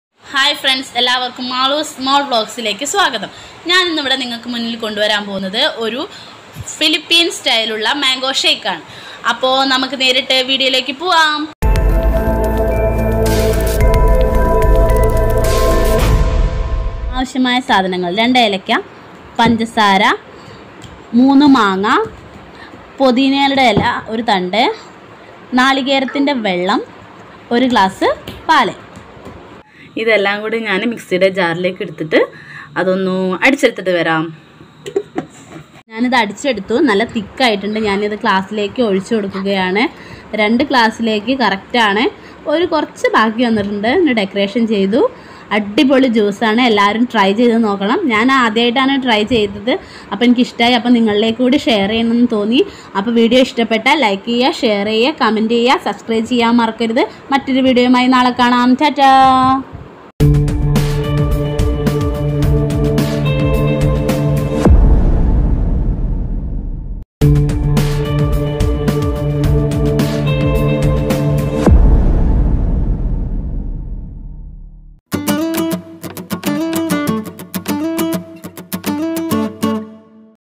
trending vlogs
hai friends this is a philipino mango shake
ingredients
1.Mangos :3 no
2.Sugar     :2 tsp
3.Mint leaf:3 or 4 no
4.Coconut water:1/2 cup
5:Milk.      :1cup
6.cardamom:2 no

 All item put in a jar & mix well then we got a delicious philipino mango shake & serve it cool thank you

Camera used : OPPO A15
Edited.            : Kinemaster
Thumbnail.    :Thumbnail maker
Music.            : Kinemaster